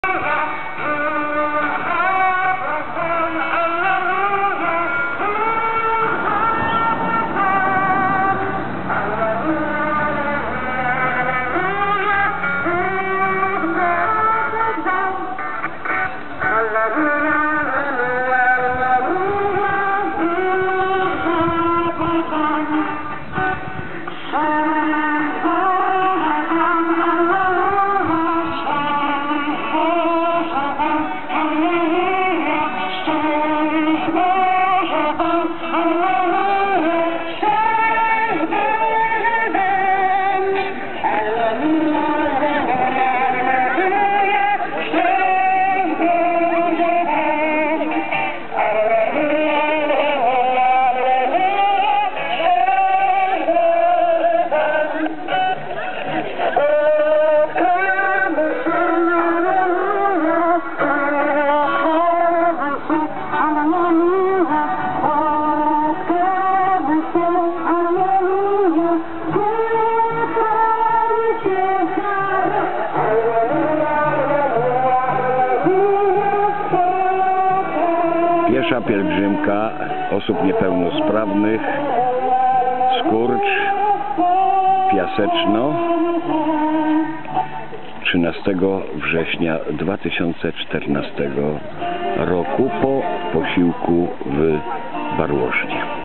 Pielgrzymka Osób Niepełnosprawnych Skórcz Piaseczno 2014 r
...po odpoczynku i posiłku przygotowanym przez panie z KGW Barłożno.